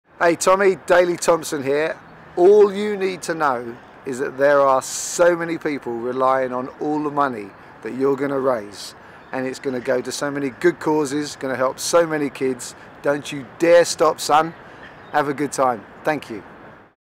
Daley Thompson Sport Relief message
Double Olympic Gold winning decathlete talks about 'all the money we can raise' for @SportRelief - if you'd like to help, you can sponsor

It's a handcycling journey that goes 1400 miles down the UK, over 20 days, with no support crew - but a trailer and tent, some superfood powders and special herbs.